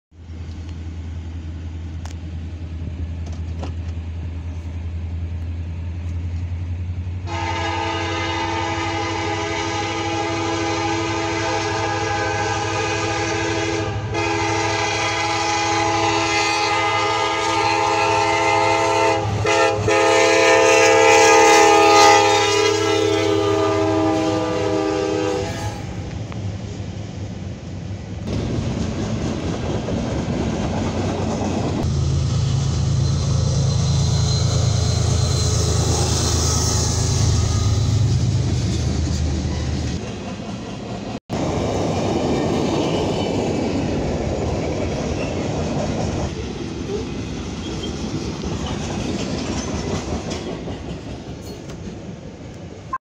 LONG AND QUICK DRIVER Leading A450 With Some rare Train cars! (CN 8808 and CN DPU 2314) Peshtigo, WI
The long and quick driver leading A450 today

Direction: (Southbound)
Time: (12:40pm)
Location: (Flame Road)